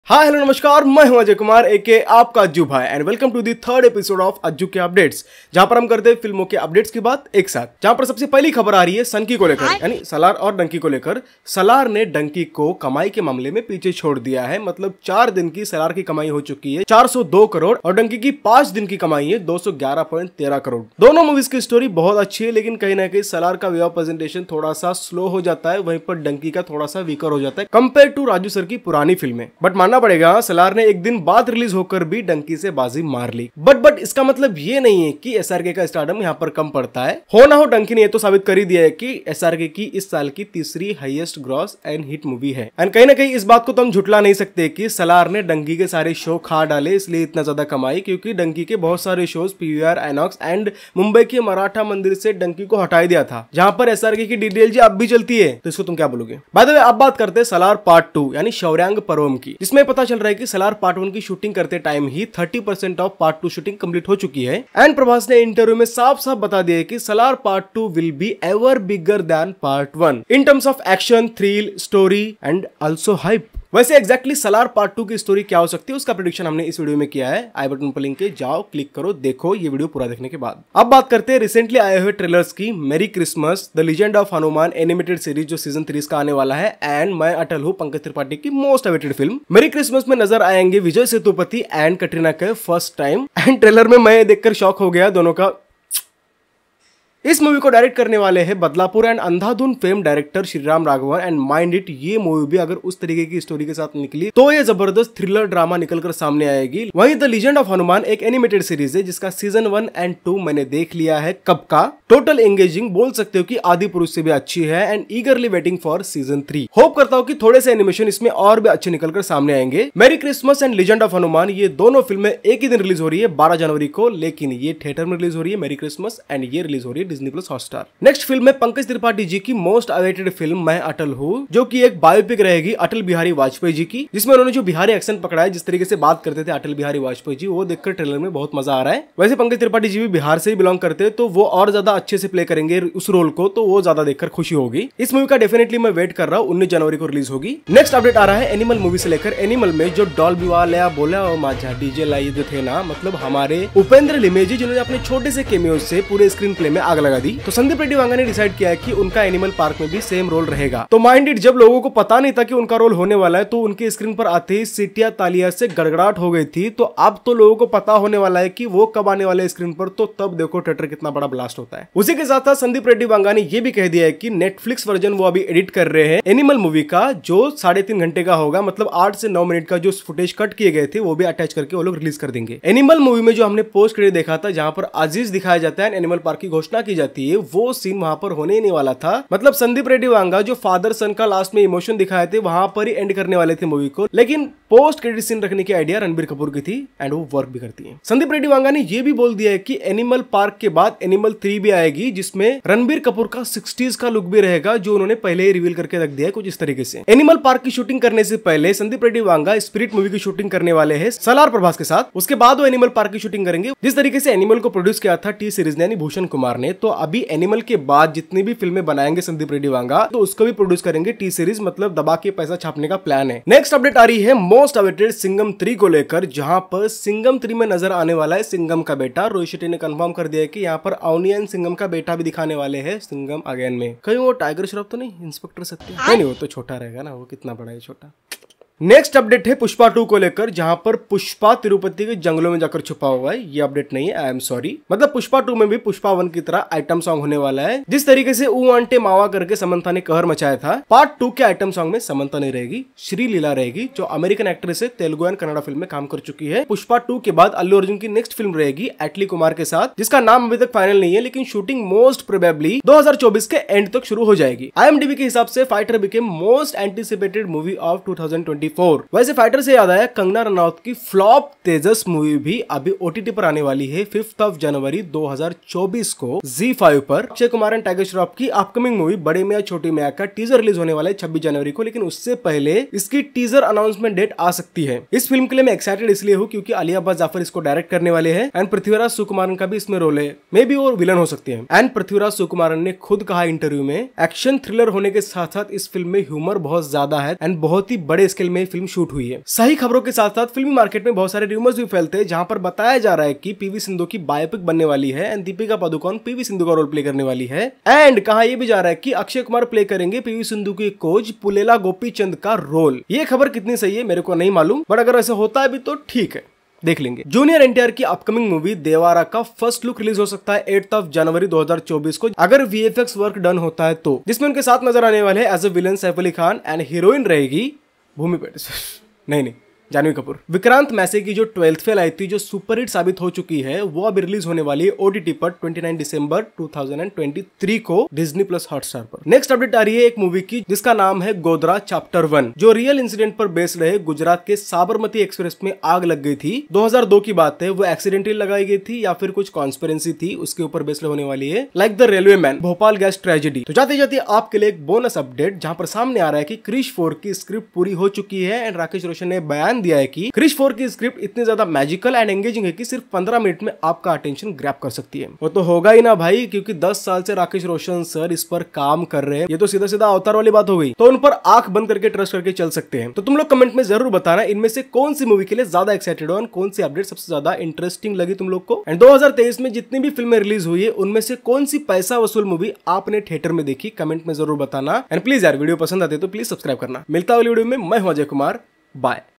Salaar Fake Collections?😰, Singham 3🤩, BMCM Teaser Big Updates🔥| Ajju K Updates Ep 03
Salaar Dominated Dunki at Box Office
Salaar vs Dunki Collections | सालार ने डंकी के पसीने छुड़ाये..!😰| Ajju K Updates Ep 03

Ajay Kumar.